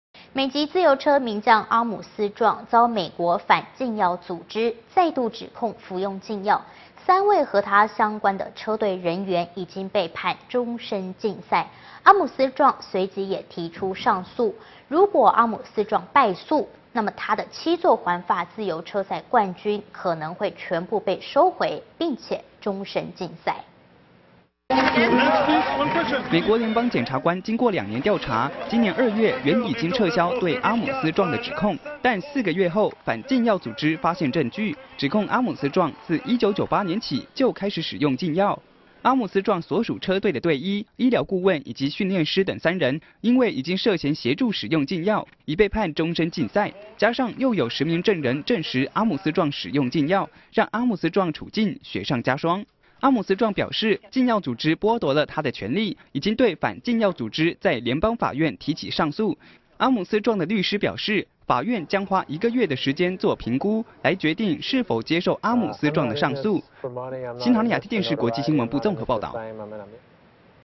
禁藥疑雲 自由車名將阿姆斯壯上訴
【新唐人亞太台2012年7月12日訊】美籍自由車名將阿姆斯壯遭美國反禁藥組織再度指控服用禁藥，三位和他相關的車隊人員已先被判終身禁賽。阿姆斯壯隨即提出上訴。如果阿姆斯壯敗訴，他的七座環法自由車賽冠軍恐將全部被收回，並終身禁賽。

美國聯邦檢察官經過兩年調查，今年二月原已撤銷對阿姆斯壯的指控；但四個月後，反禁藥組織(USADA)發現證據，指控阿姆斯壯自1998年起就開始使用禁藥。

阿姆斯壯所屬車隊的隊醫、醫療顧問、及訓練師等三人已因涉協助使用禁藥，已被判終生禁賽，加上又有十名證人證實阿姆斯壯使用禁藥，讓阿姆斯壯處境雪上加霜。

阿姆斯壯表示，禁藥組織剝奪了他的權利，已經對反禁藥組織在聯邦法院提起上訴。阿姆斯壯的律師表示，法院將花一個月的時間做評估，已決定是否接受阿姆斯壯的上訴。

新唐人亞太電視 國際新聞部 綜合報導

其他推薦:
看動態網：曉天下事